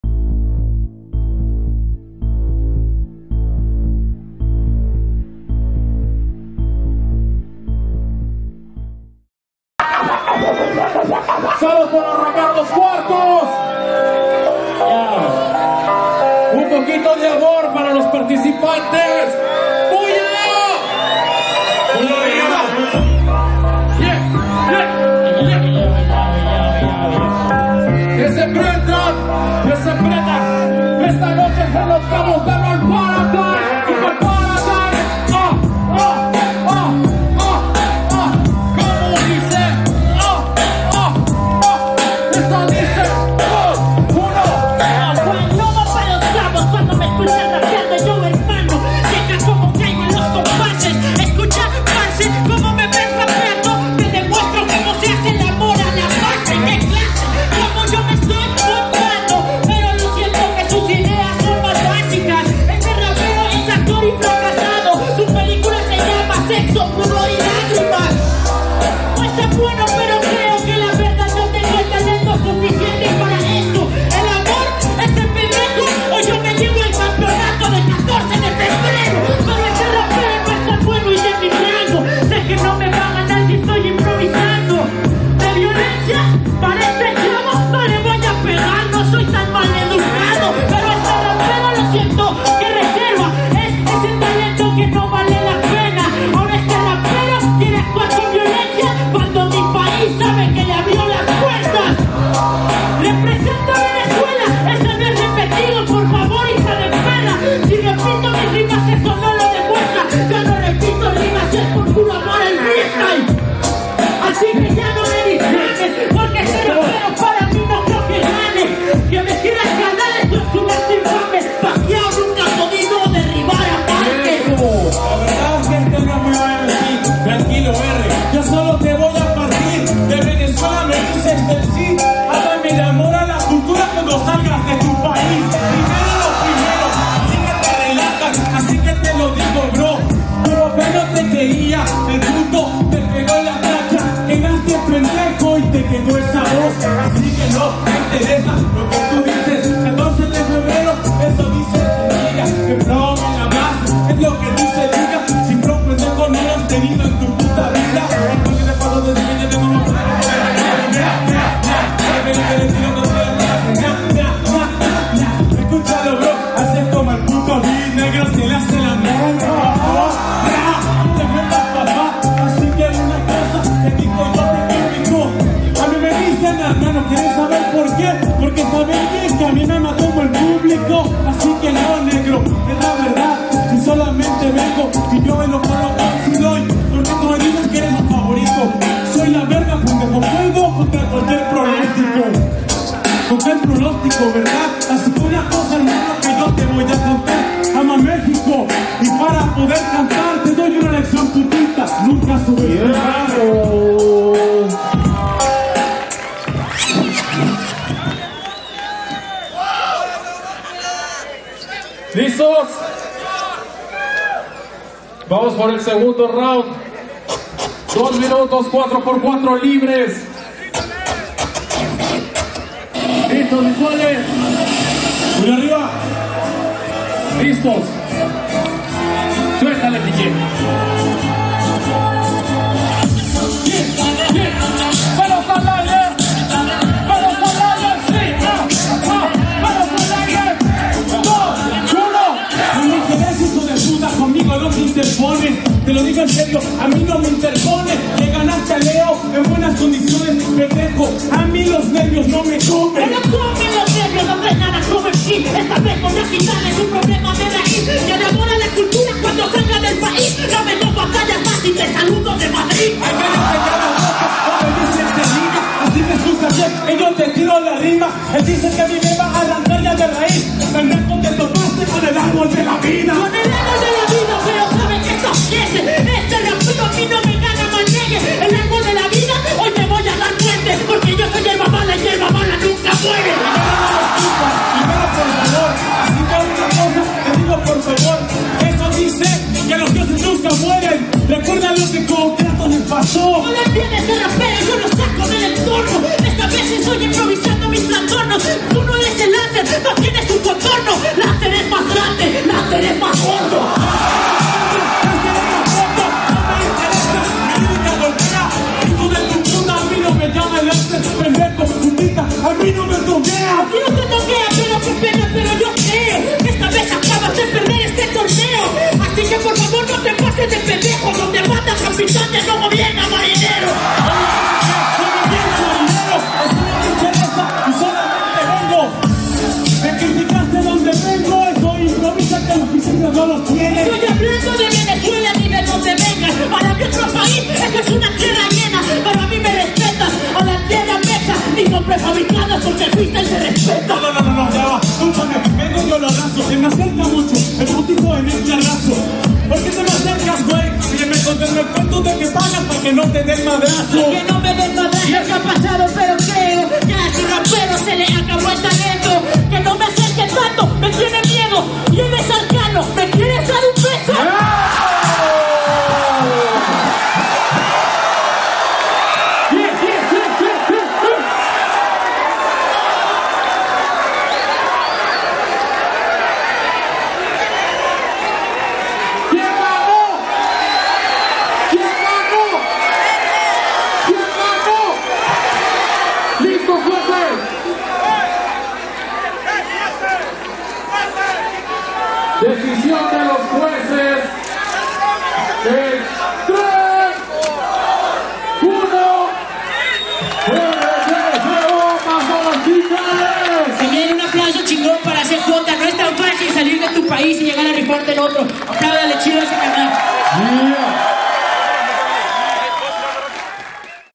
CJ vs RDGO - Cuartos de Final Verbal Paradise Los Cabos 2019
FINAL NACIONAL VERBAL PARADISE 2019 CELEBRADA EN LOS CABOS, B.C.S.

COMPETIDORES LOCALES Y NACIONALES SE ENFRENTARON EN EL TORNEO “VERBAL PARADISE” FREESTYLE BATTLE LEAGUE PARA GANAR SU PASE A LA INTERNACIONAL DE 420 BACKYARD BATTLE EN MADRID, ESPAÑA.
COMPETIDORES:

1.- AEMA
2.- SKIPER
3.- JULIO GASGA
4.- HASSEL
5.- HACKER
6.- ERICK D
7.- TITO DIAZ
8.- LEO MC
9.- RDGO
10.- YDEAS
11.- SKEL
12.- F-NUEVE
13.- CJ
14.- DAX
15.- CHUPIN
16.- AZKILLAH